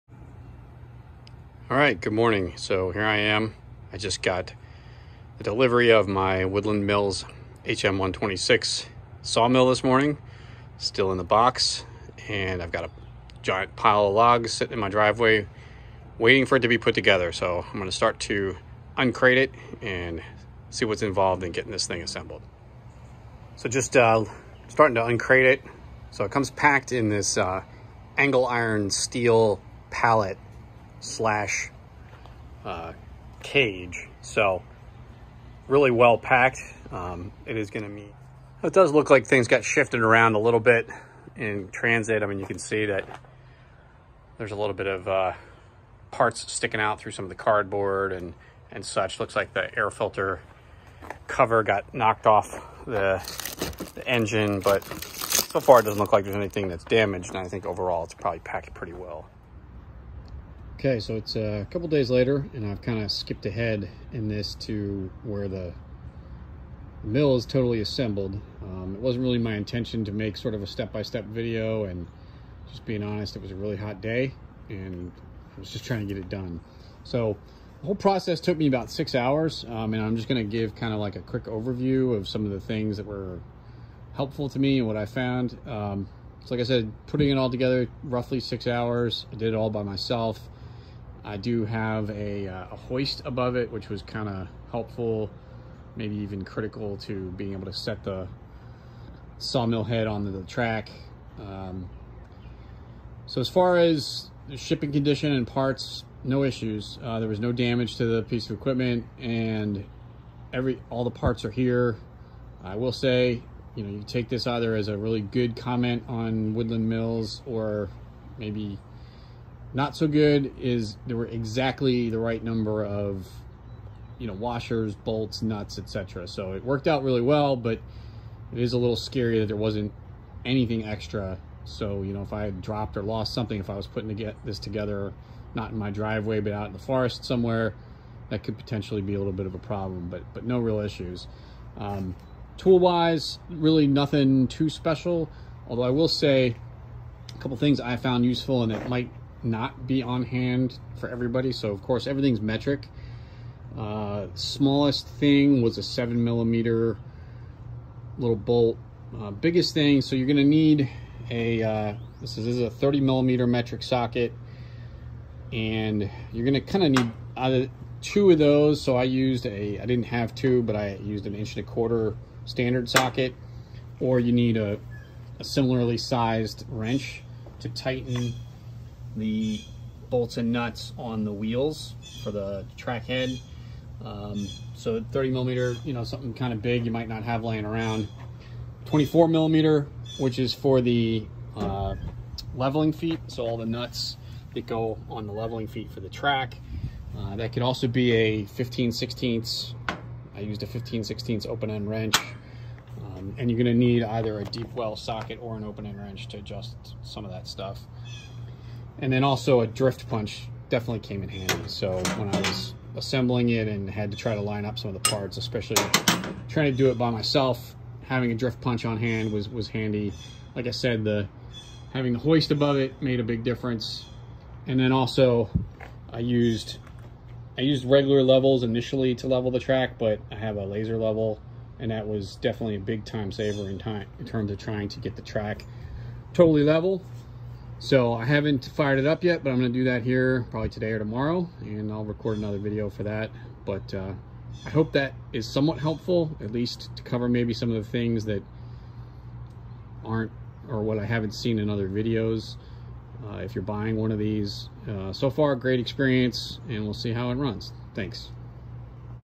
Woodland Mills HM126 Sawmill Unpacking and Assembly Tips
This is a short video about what you can expect and what you will need when you get your Woodland Mills sawmill. It's not meant to be a step-by-step instructional video, just some observations and comments on what you'll need to unpack and assemble it. I hope you find it helpful.